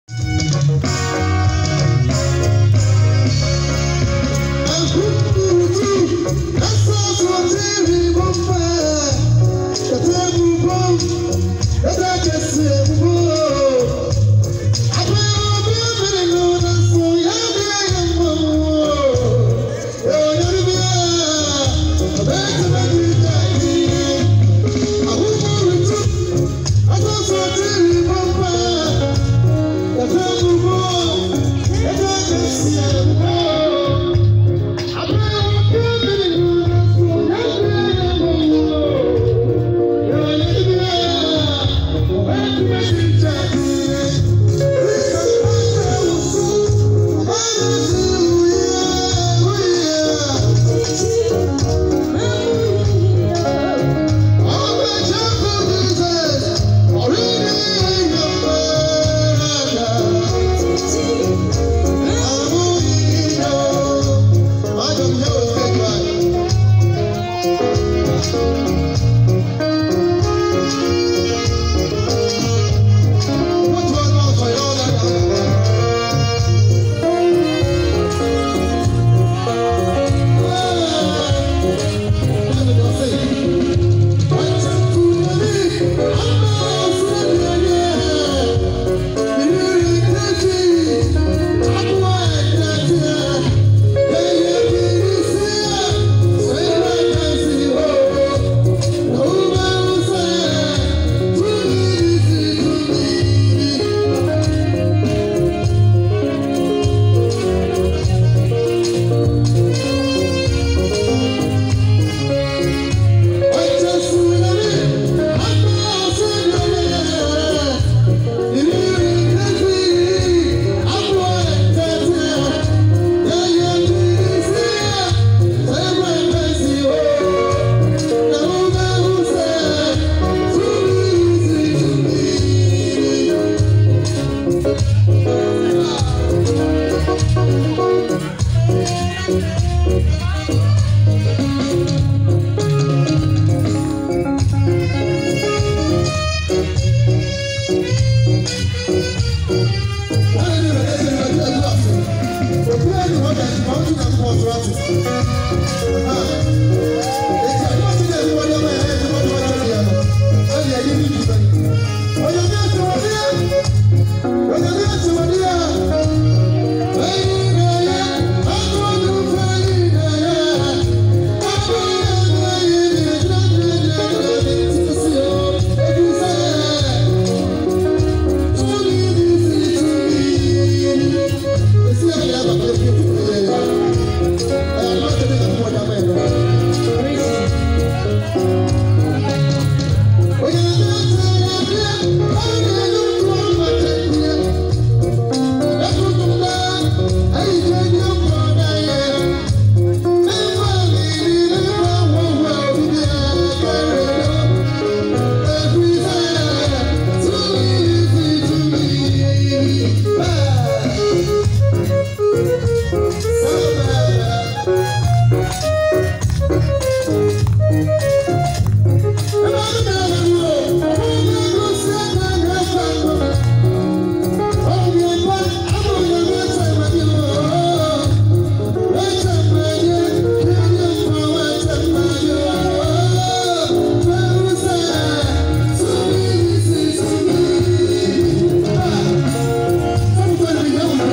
Odehyieba Priscilla's Manager Singing to Welcome Guests at Her Birthday Party
PLEASE NOTE: Unauthorized upload of this video unto any YouTube channel is prohibited.